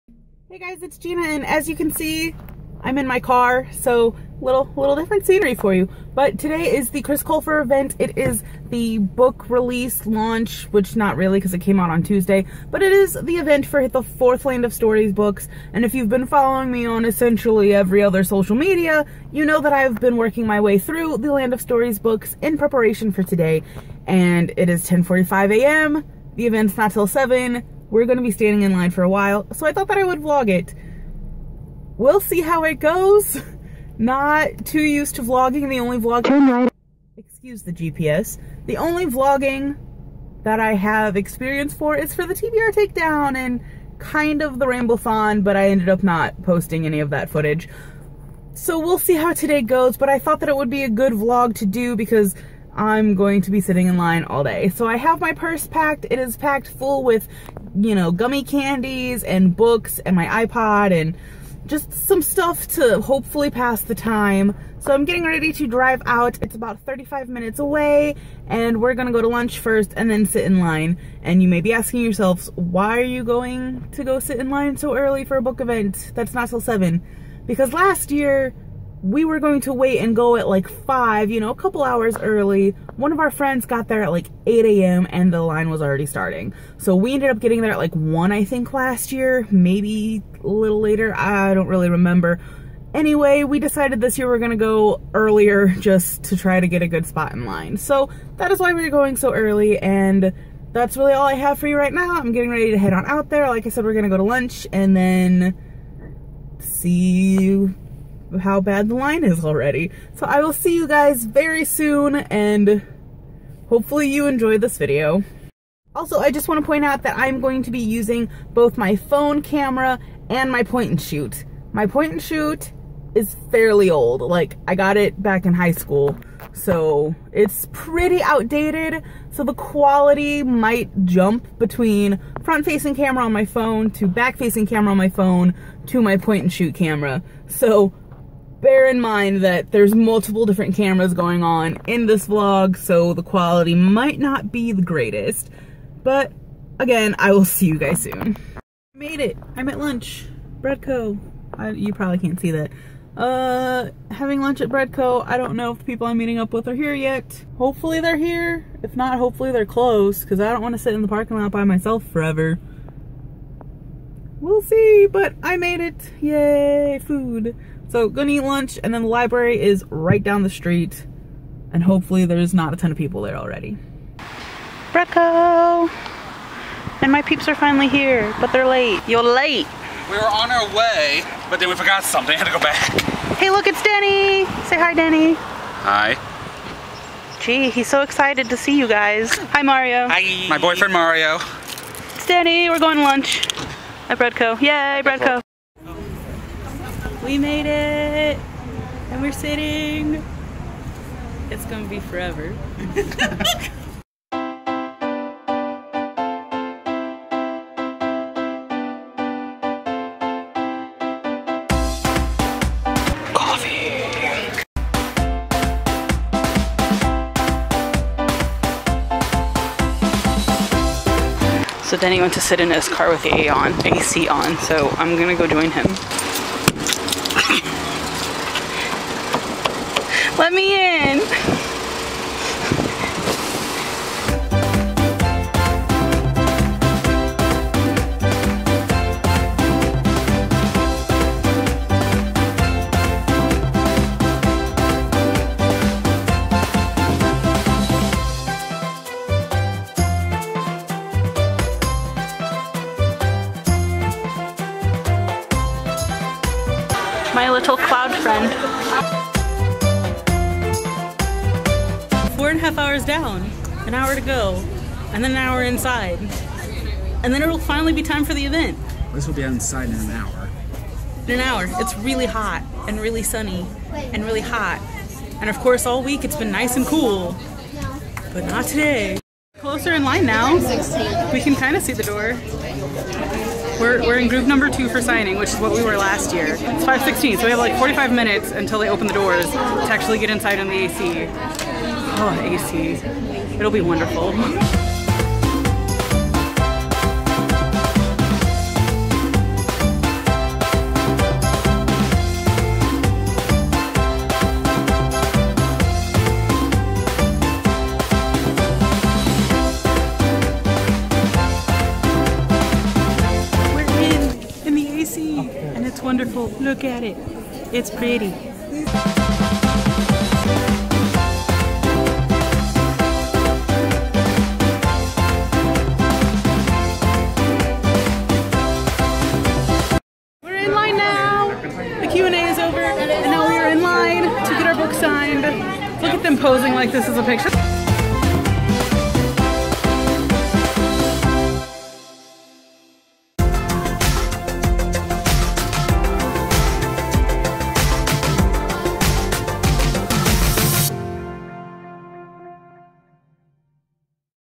Meeting Chris Colfer | July 2015
On Saturday, July 11th I got to meet the wonderful Chris Colfer for the second time at an event for his novel, The Land of Stories: Beyond the Kingdom; the fourth novel in his Land of Stories series. We went early to get a good spot in line and sat in extreme heat, but in the end it was a fun day :) Enjoy!!

SOCIAL MEDIA LINKS!! :D :D